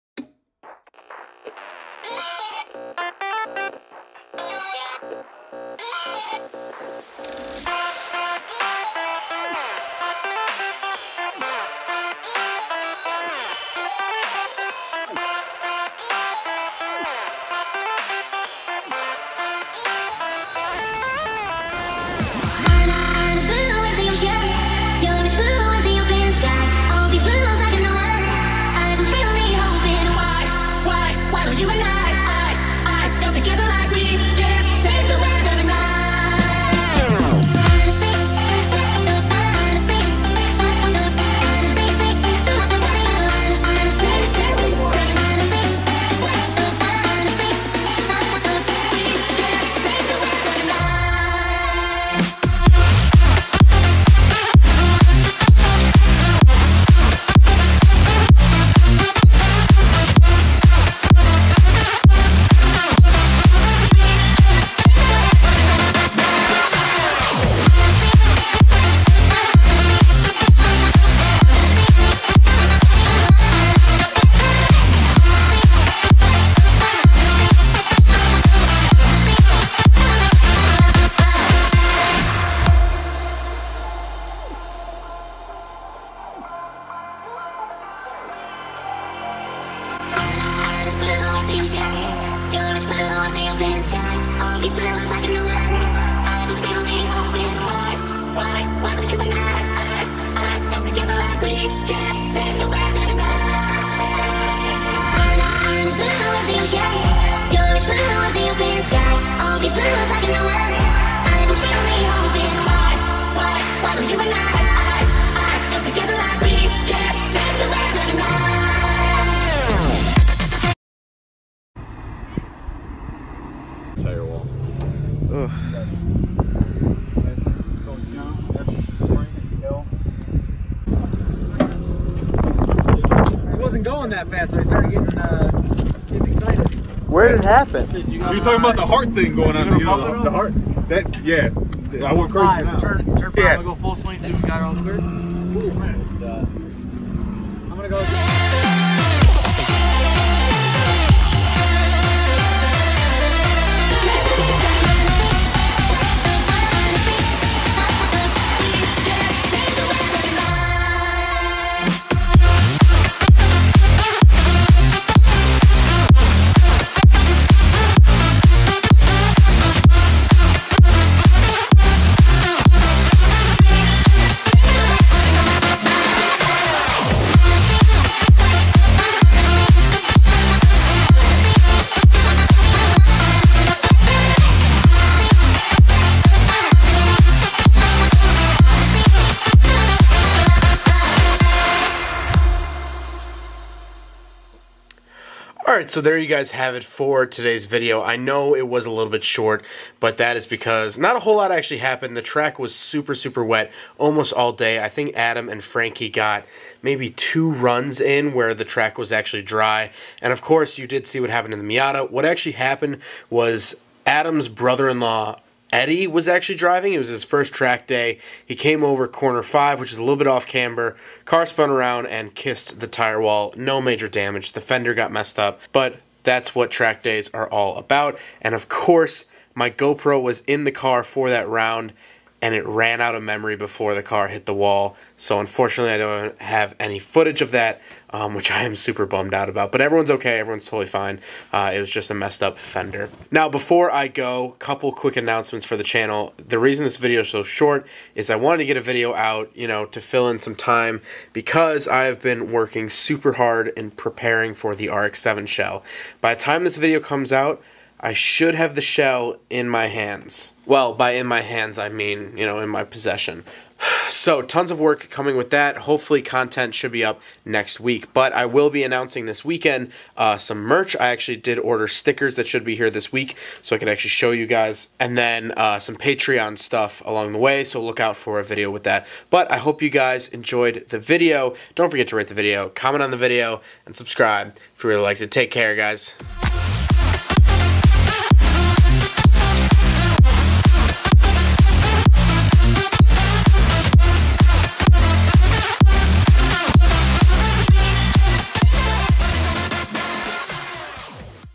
The Art of Racing (Mazdas) in The Rain
I head out to Autobahn Country Club to watch Adam (Black Miata) and Frankie (White Rx-7) battle the rain on the track!